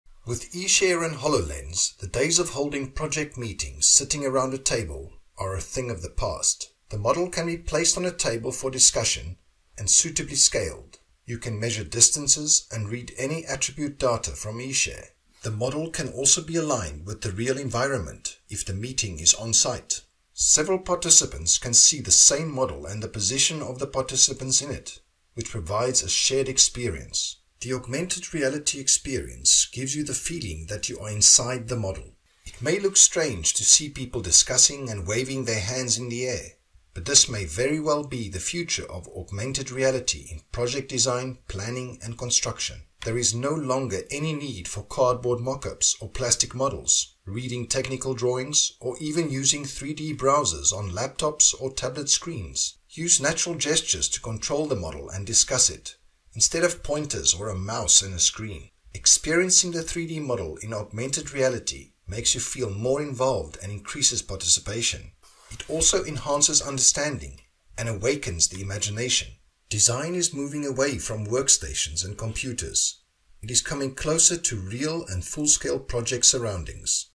CADMATIC eShare for HoloLens
eShare for HoloLens is an app that allows the user to interface Microsoft HoloLens* with CADMATIC eShare.